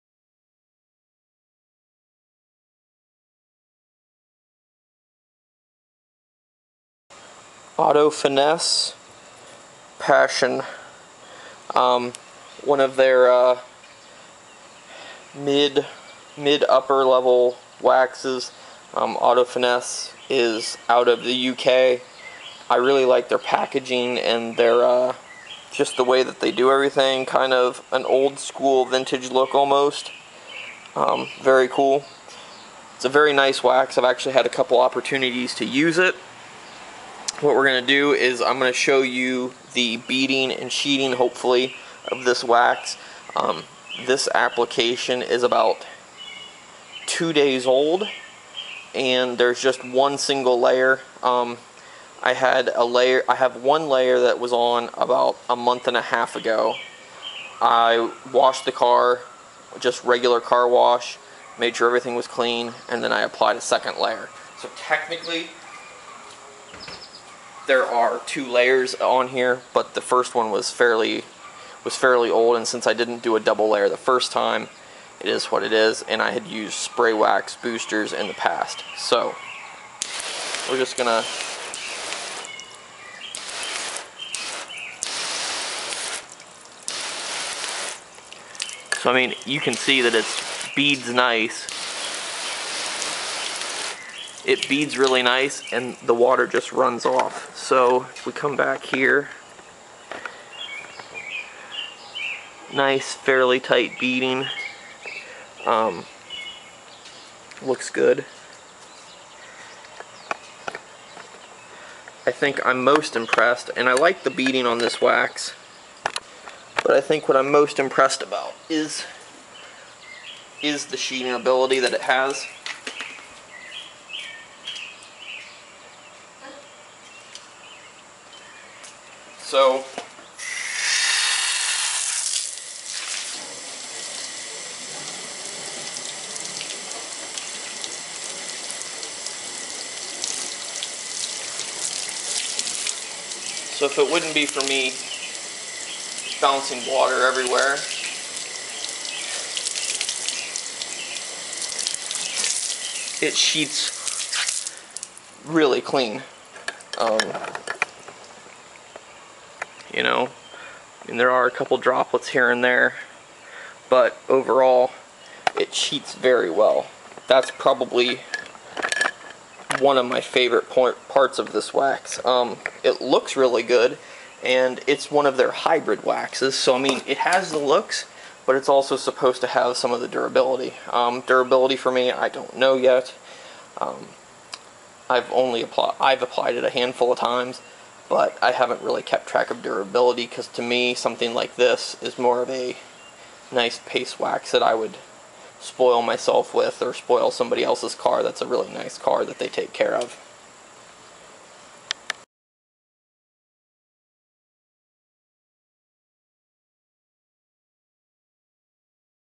Auto Finesse Passion: Beading & Sheeting
Testing the Beading & Sheeting of Auto Finesse Passion.